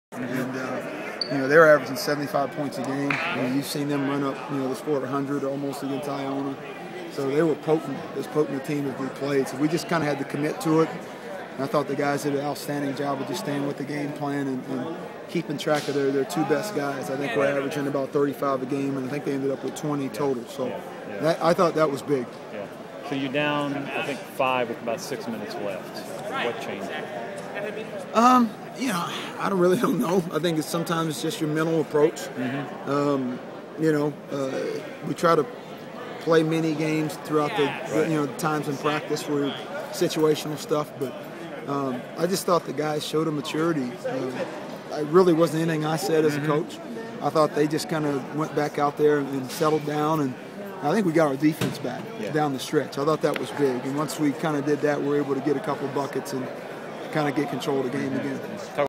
Charlotte 49ers Mens Basketball Major postgame comments at Saint Joseph's
Charlotte 49ers Head coach Alan Major postgame comments at Saint Joseph's.